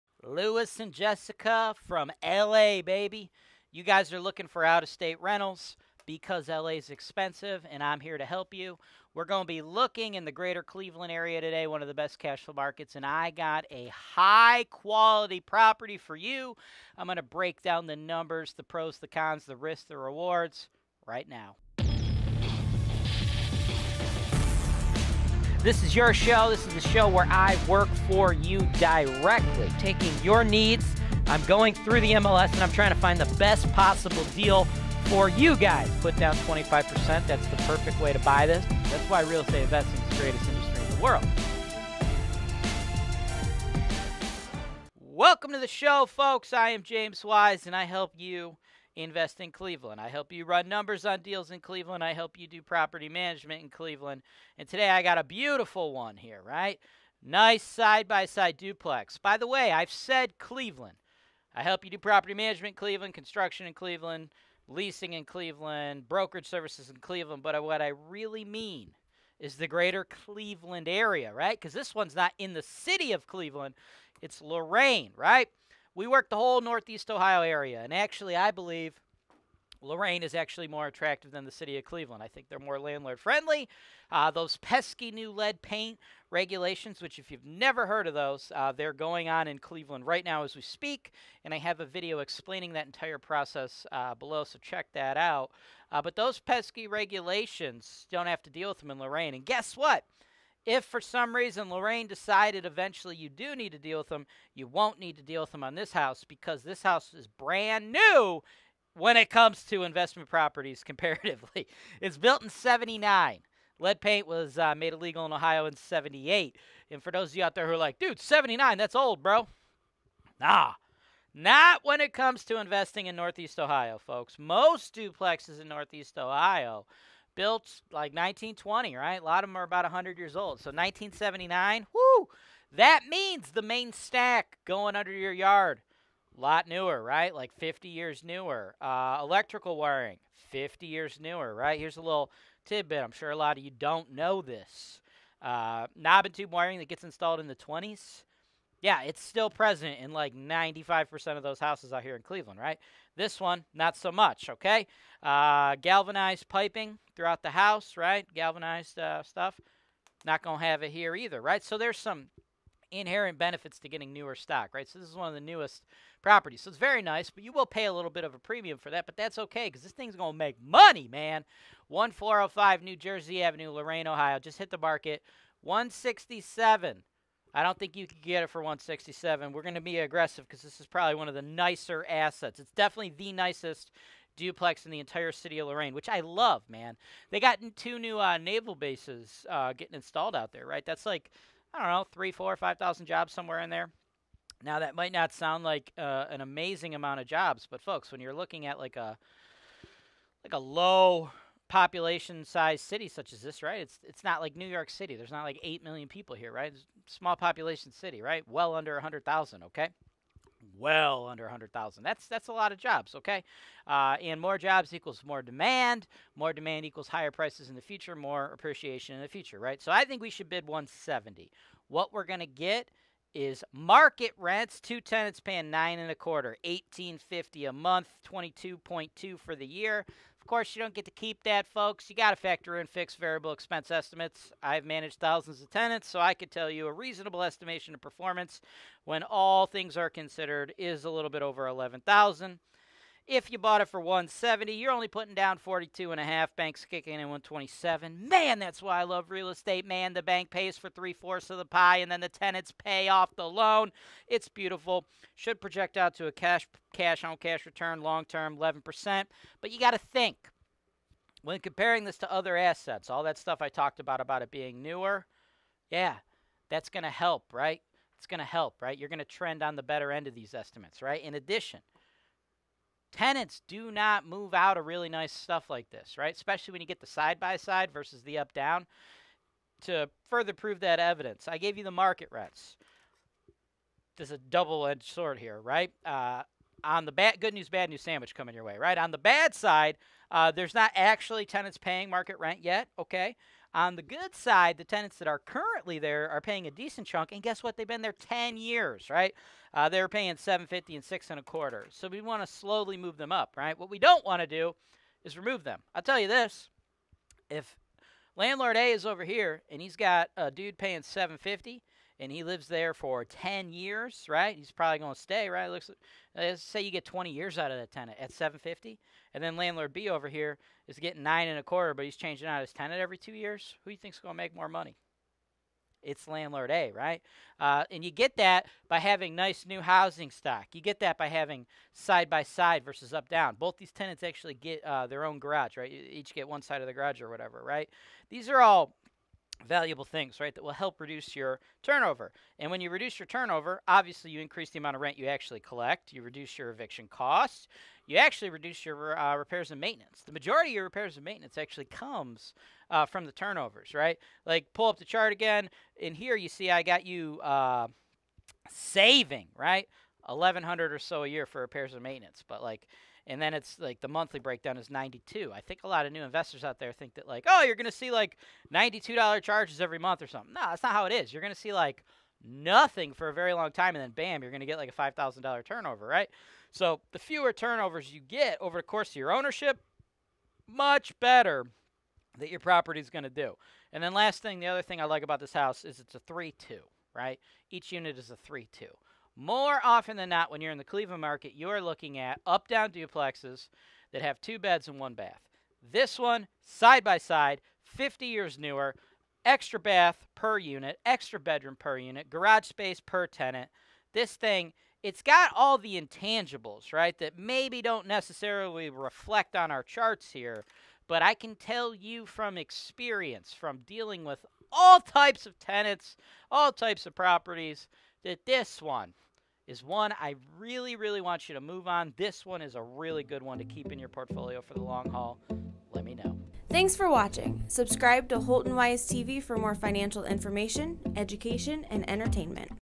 Helping Lou from Los Angeles Buy Affordable Housing | MLS Search & Analysis 1,888 - 1405 New Jersey
Buying affordable housing and then renting it out to tenants is a great way to make some extra money. The thing is, depending on where you live, there might not be any affordable housing that is actually affordable. For example, the Los Angeles area is incredibly expensive. Investor who are interested in buying affordable housing in the LA area are often priced out due to the incredibly high cost of housing throughout California. Many of these investors end up buying properties out of state instead. Louis and Jessica from Los Angeles, California are looking to invest in affordable housing in Cleveland, Ohio. During the 1,888th episode of The MLS Search & Analysis Show, affordable housing expert James Wise is doing a real estate analysis on a side by side duplex in the greater Cleveland, Ohio area.

📺 HELPFUL RESOURCES
Holton-Wise FAQ